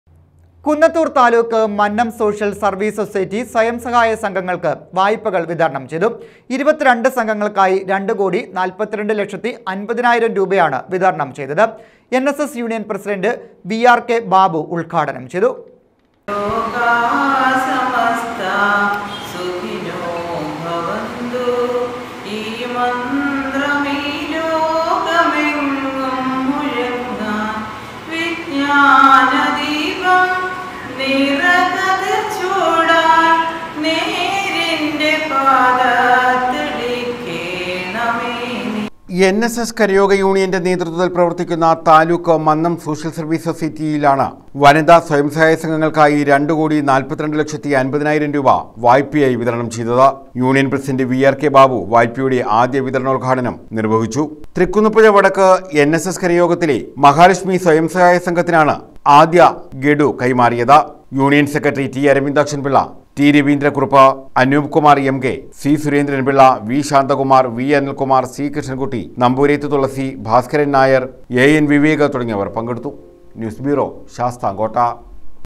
മന്നം സോഷ്യല്‍ സര്‍വ്വീസ് സൊസൈറ്റി സ്വയം സഹായ സംഘങ്ങള്‍ക്ക് വായ്പകള്‍ വിതരണം ചെയ്തു
കുന്നത്തൂര്‍ താലൂക്ക് മന്നം സോഷ്യല്‍ സര്‍വ്വീസ് സൊസൈറ്റി സ്വയം സഹായ സംഘങ്ങള്‍ക്ക് വായ്പകള്‍ വിതരണം ചെയ്തു. 22 സംഘങ്ങള്‍ക്കായി 2 കോടി 42,50000 രൂപയാണ് വിതരണം ചെയ്തതത്. എന്‍.എസ്.എസ് യൂണിയന്‍ പ്രസിഡന്റ് വി.ആര്‍.കെ ബാബു ഉദ്ഘാടനം ചെയ്തു.